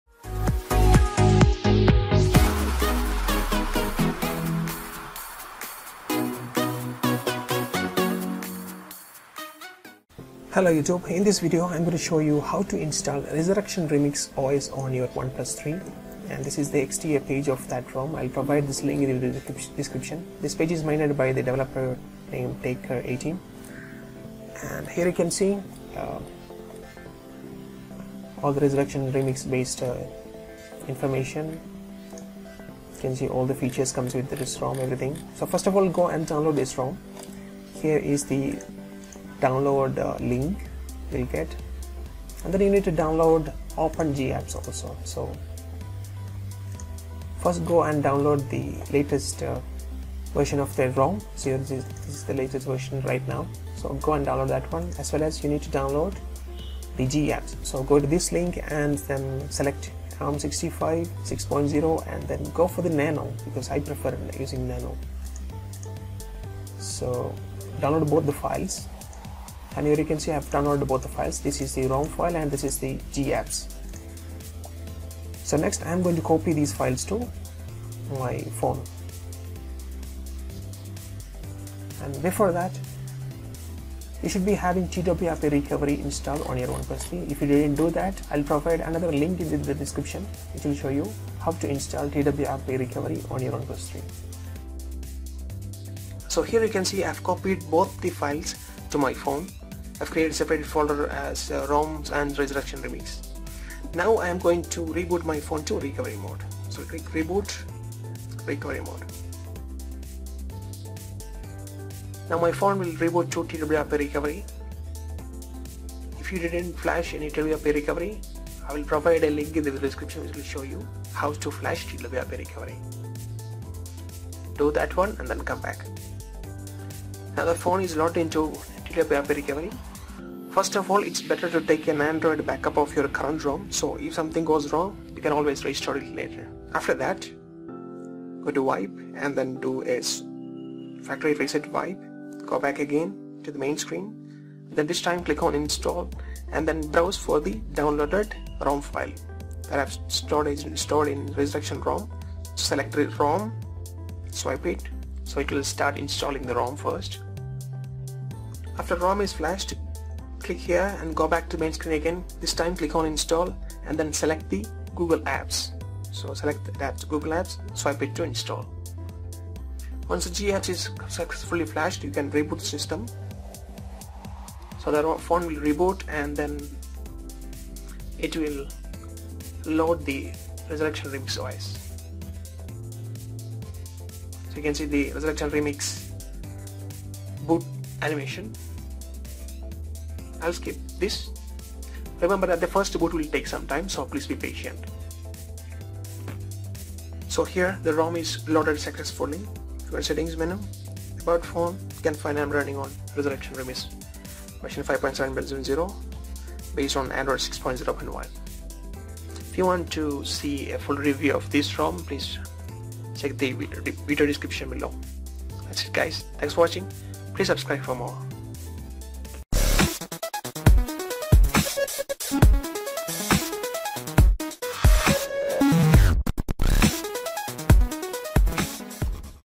How to Install Resurrection Remix ROM on OnePlus 3
This video will show you How to How to Install Resurrection Remix ROM on OnePlus 3.